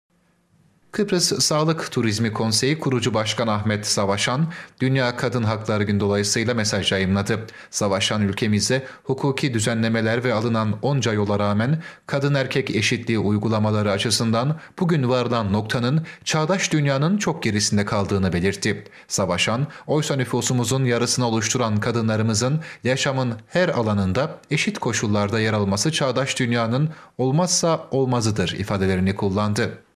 SAVAŞAN: “DÜNYANIN ÇOK GERİSİNDEYİZ”
Kıbrıs Sağlık Turizmi Konseyi Kurucu Başkanı Ahmet Savaşan, Dünya Kadın Hakları Günü dolayısıyla mesaj yayımladı. Savaşan, Ülkemizde hukuki düzenlemeler ve alınan onca yola rağmen; kadın-erkek eşitliği uygulamaları açısından bugün varılan noktanın çağdaş dünyanın çok gerisinde kaldığını  belitti. Savaşan, “Oysa nüfusumuzun yarısını oluşturan kadınlarımızın yaşamın her alanında eşit koşullarda yer alması çağdaş Dünya’nın olmazsa olmazıdır” ifadelerini kullandı.